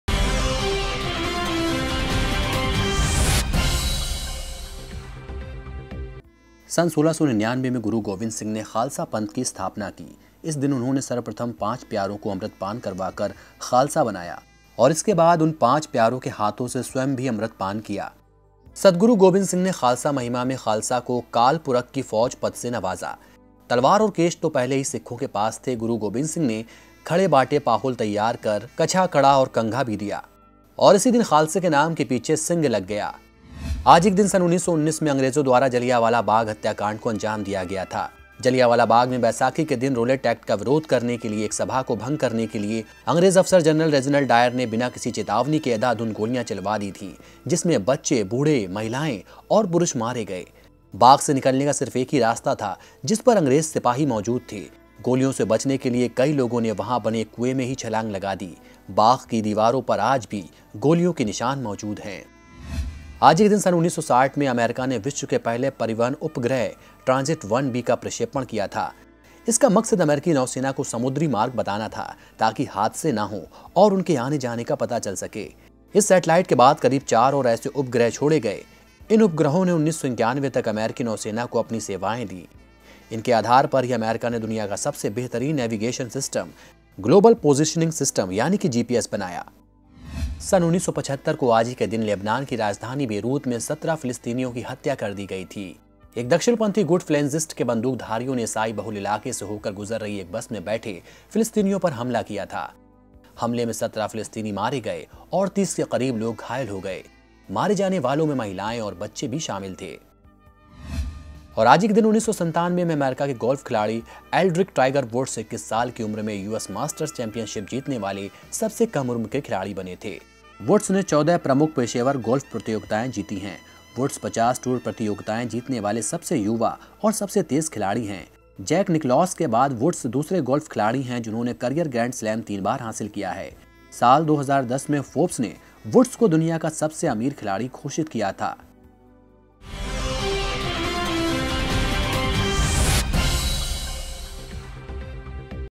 DBLIVE | 13 April | Taarikh Gawah Hai
1..  सन् 1699 में गुरु गोबिंद सिंह ने खालसा पंथ की स्थापना की। इस दिन उन्होंने सर्वप्रथम पाँच प्यारों को अमृतपान करवा कर खालसा बनाया और उसके बाद उन पाँच प्यारों के हाथों से स्वयं भी अमृतपान किया। सतगुरु गोबिंद सिंह ने खालसा महिमा में खालसा को "काल पुरख की फ़ौज" पद से नवाज़ा। तलवार और केश तो पहले ही सिखों के पास थे,  गुरु गोबिंद सिंह ने "खंडे बाटे की पाहुल" तैयार कर कछा,  कड़ा और कंघा भी दिया|  इसी दिन खालसे के नाम के पीछे "सिंह" लग गया।

2..  सन् 1919 में अंग्रेजों द्वारा जलियांवाला बाग हत्याकांड को अंजाम दिया गया था। जलियांवाला बाग में बैसाखी के दिन रोलेट एक्ट का विरोध करने के लिए एक सभा को भंग करने के लिए अंग्रेज अफसर जनरल रेजीनल्ड डायर ने बिना किसी चेतावनी के अंधाधुंध गोलियां चलवा दीं थीं, जिसमें बच्चे, बूढ़े,  महिलाएं और पुरुष मारे गए। बाग से निकलने का सिर्फ एक ही रास्ता था, जिस पर अंग्रेज सिपाही मौजूद थे। गोलियों से बचने के लिए कई लोगों ने वहां बने एक कुएं में ही छलांग लगा दी थी। बाग की दीवारों पर आज भी गोलियों के निशान मौजूद हैं।

3..  सन् 1960 में अमेरिका ने विश्व के पहले परिवहन उपग्रह 'ट्रांजिट-1 बी'  का प्रक्षेपण किया था। इसका मकसद अमेरिकी नौसेना को समुद्री मार्ग बताना था, ताकि हादसे न हों और उनके आने-जाने का पता चल सके। इस सैटेलाइट के बाद करीब चार और ऐसे उपग्रह छोड़े गए। इन उपग्रहों ने 1991 तक अमेरिकी नौसेना को अपनी सेवाएं दीं । इनके आधार पर ही अमेरिका ने दुनिया का सबसे बेहतरीन नेविगेशन सिस्टम ‘ग्लोबल पोजिशनिंग सिस्टम-जीपीएस’  बनाया।

4..  सन् 1975 को लेबनान की राजधानी बेरुत में 17 फ़िलस्तीनियों की हत्या कर दी गई थी। एक दक्षिणपंथी गुट 'फ़लेन्जिस्ट' के बंदूक धारियों ने ईसाई बहुल इलाक़े से होकर गुज़र रही एक बस में बैठे फ़िलस्तीनियों पर हमला किया था।  हमले में 17 फिलस्तीनी मारे गए और 30 के क़रीब लोग घायल हो गए। मारे जाने वालों में महिलाएं और बच्चे भी शामिल थे।